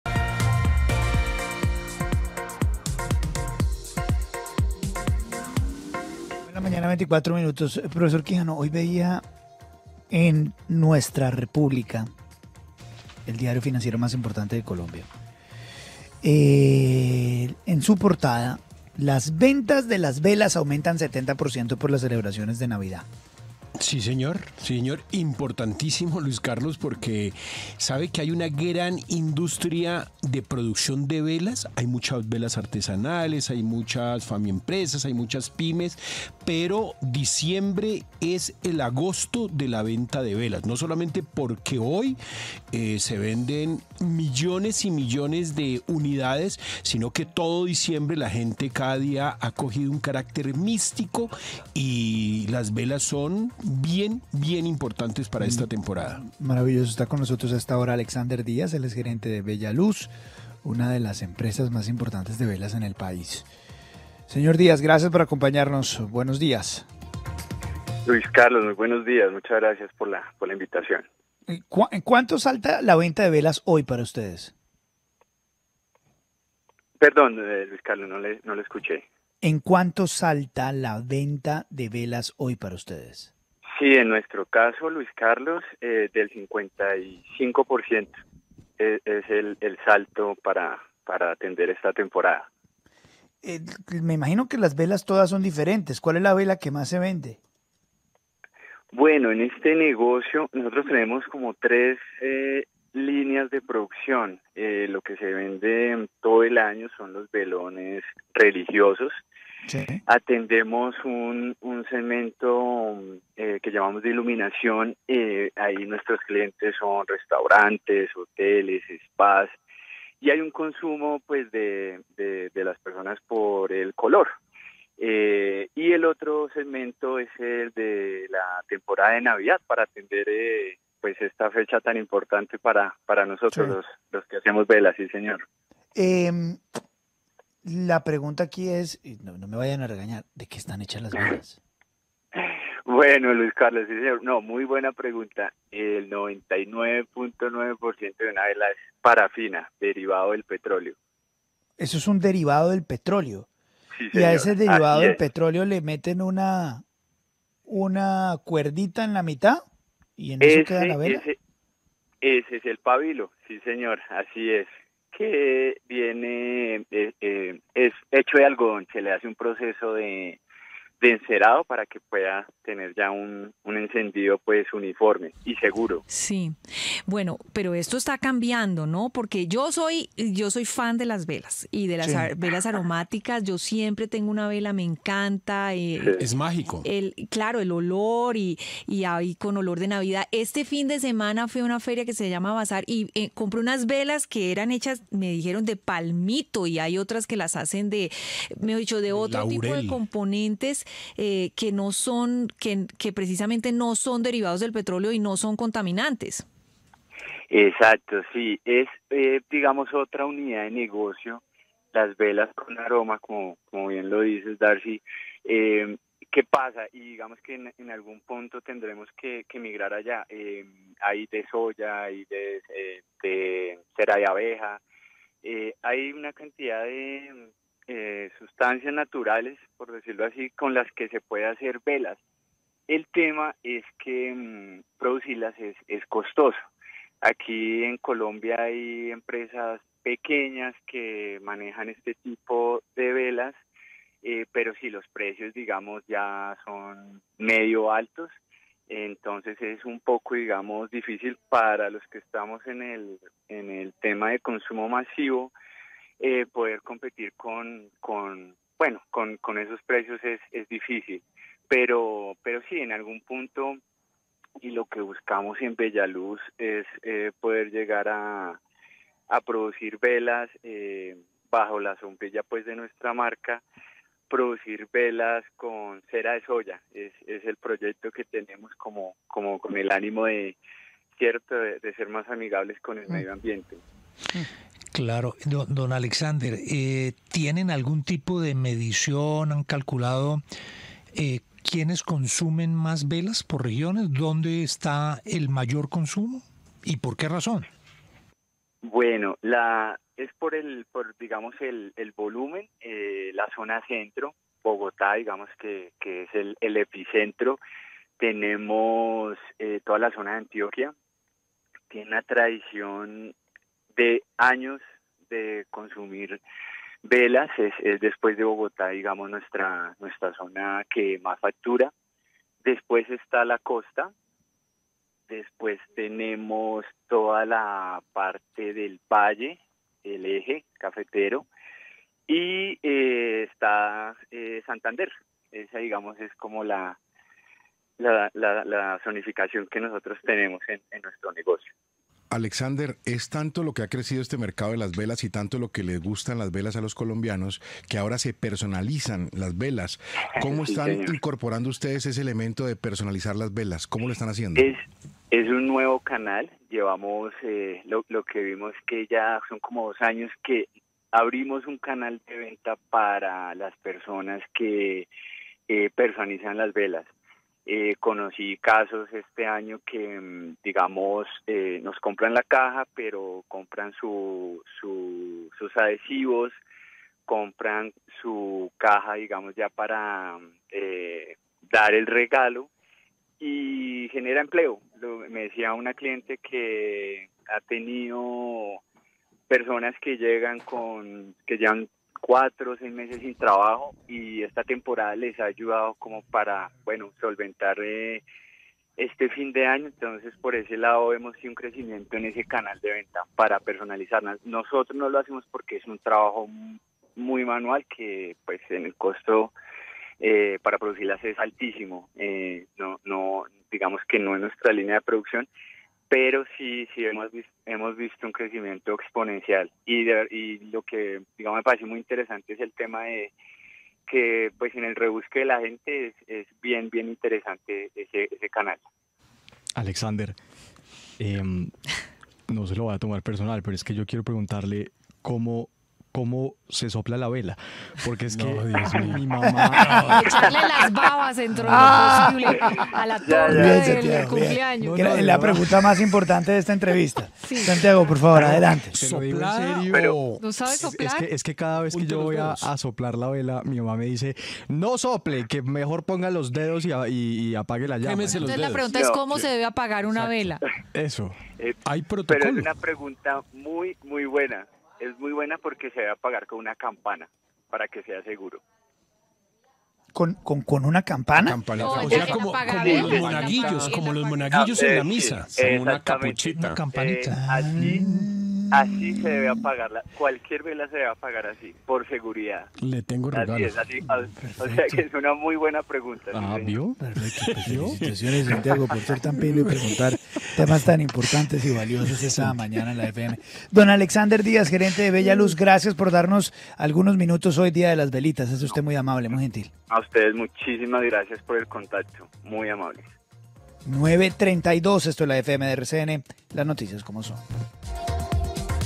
Este es el aumento real de la venta de velitas en Navidad
En entrevista con la empresa de velas, Bella Luz supimos como es la temporada de Diciembre previo a la Navidad. Acá la entrevista completa